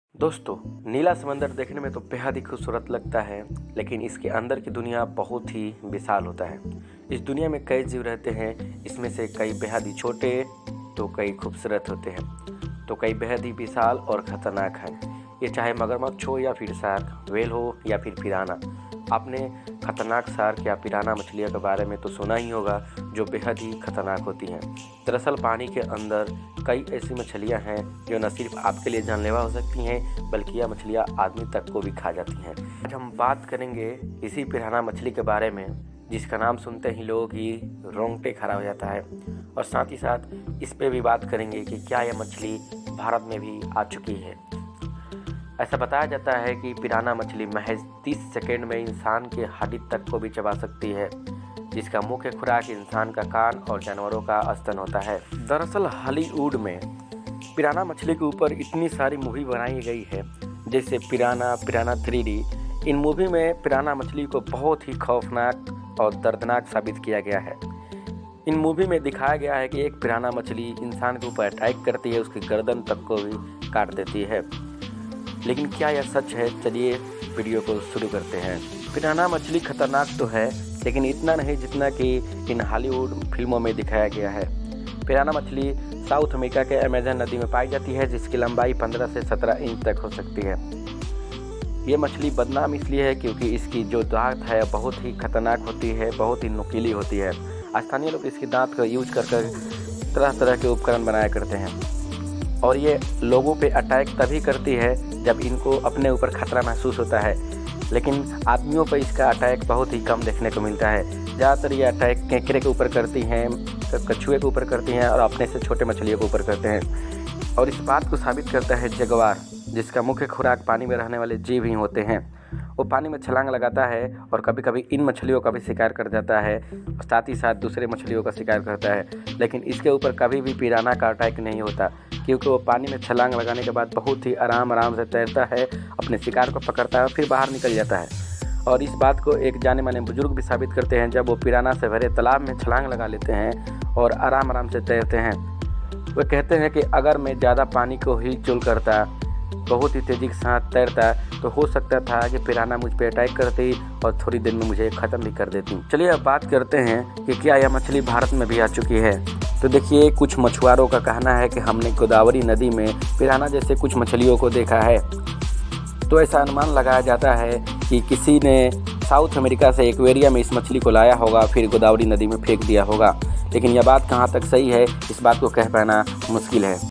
Pirhana fish (आदमखोर मछली)
Is video me  maine aapko bataya hai  pirhana machhali ke kuchh khas batein ki kya pirhana machhali sachmuch Khatarnak hoti hain kya pirhana machhali aadami ko khati hain ya sirf afwah hai.
pirhana fish
pirhana fish in hindi
pirhana fish attack
all about pirhana machhli